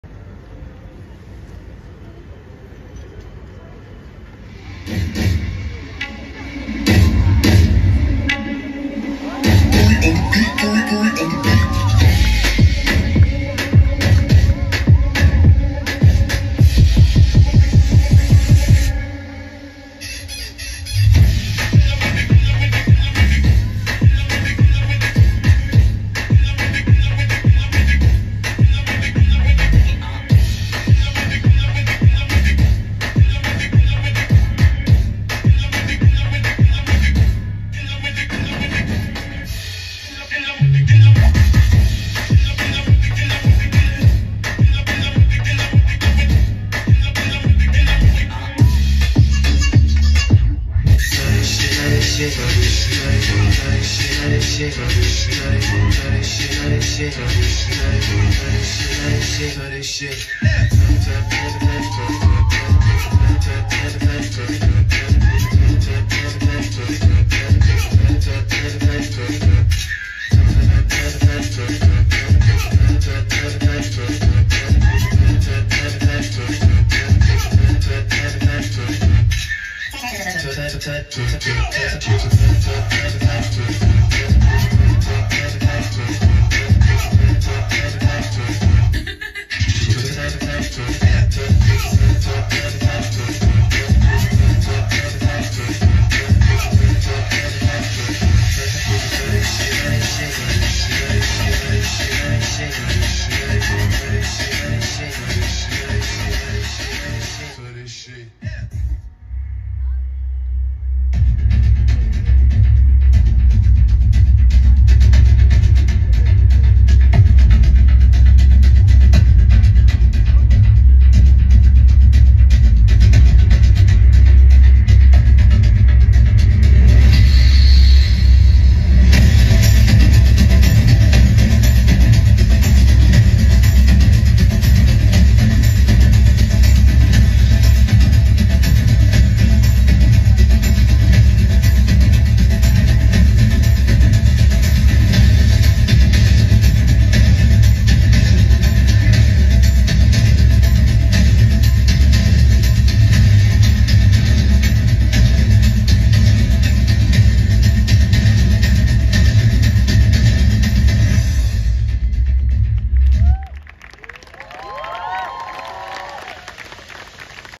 Round square closing ceremony Dance
This is performed by the primary students of HPS by the slogan ‘Inhale Courage Exhale Fear’